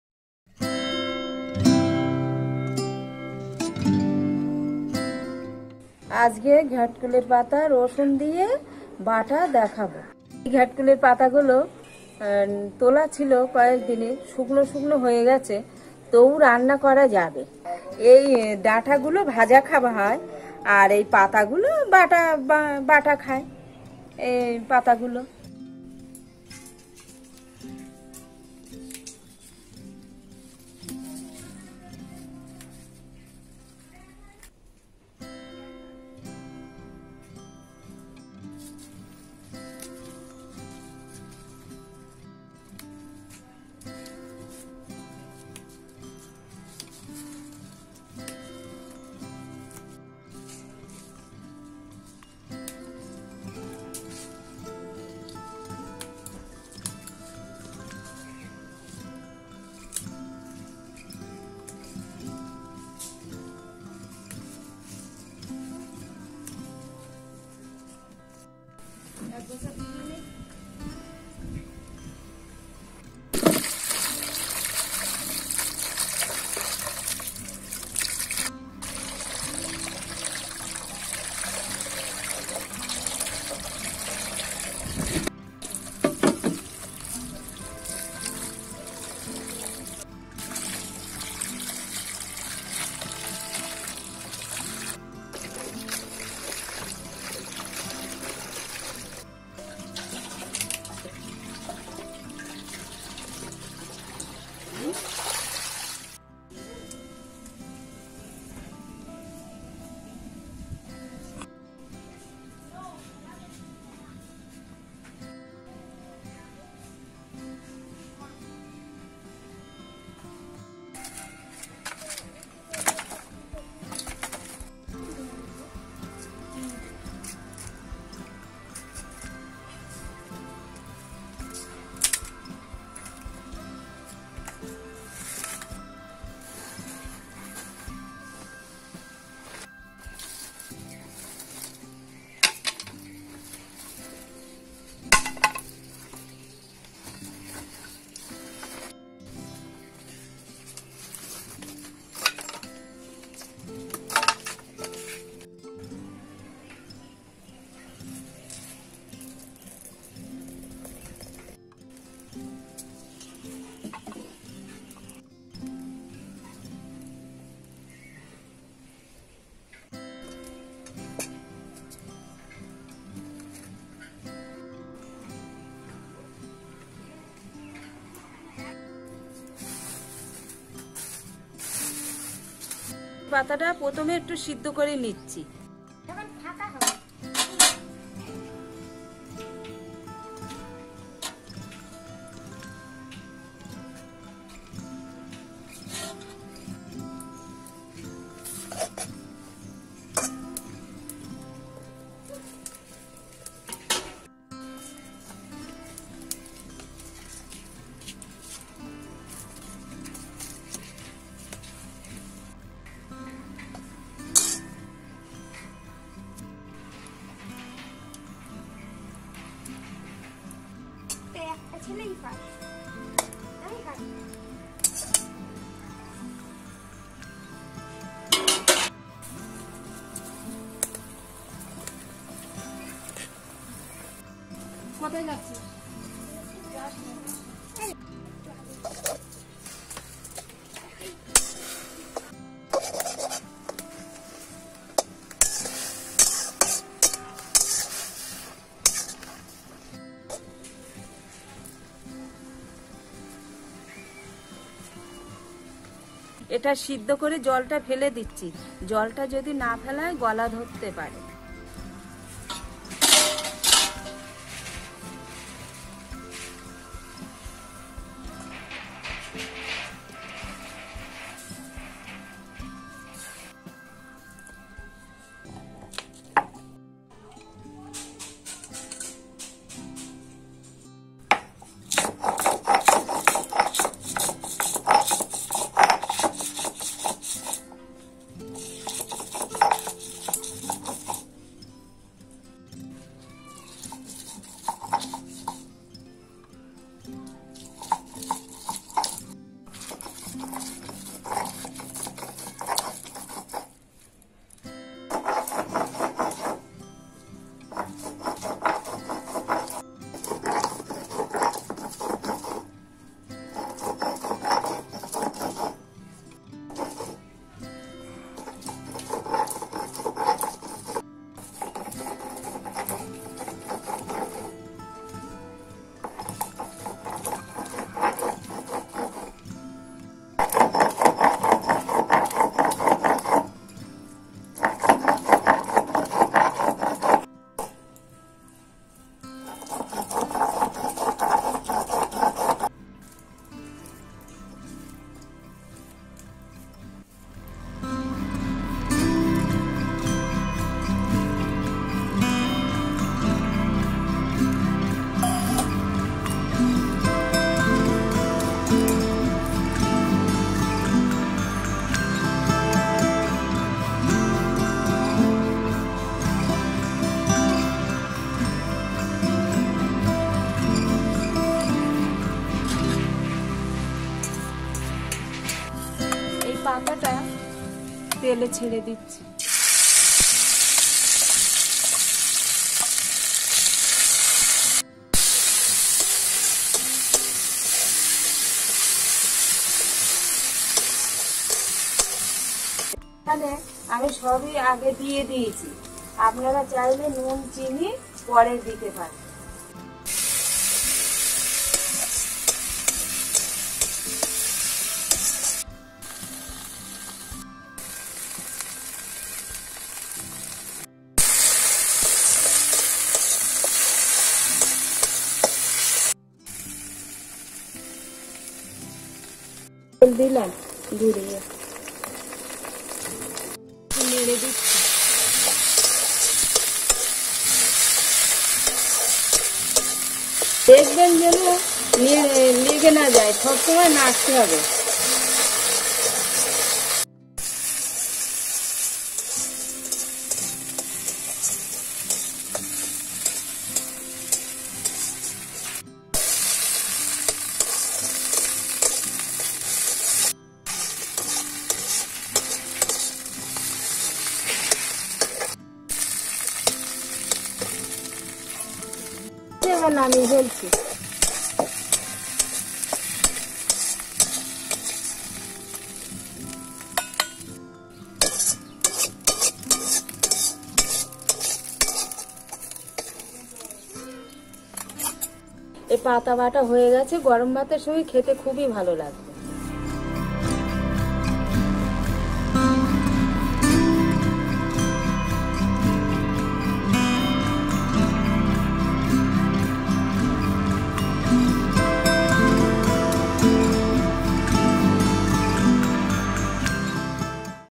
Ghatkol Pata Bata Rosun Diye - ঘ্যাটকোল বা খারকোল পাতা বাটা
Ghatkol pata bata rosun diye recipe or kharkol pata bata recipe in Bengali village style By Indian Village Cooking, ঘ্যাটকোল পাতা বাটা,খারকোল পাতা বাটা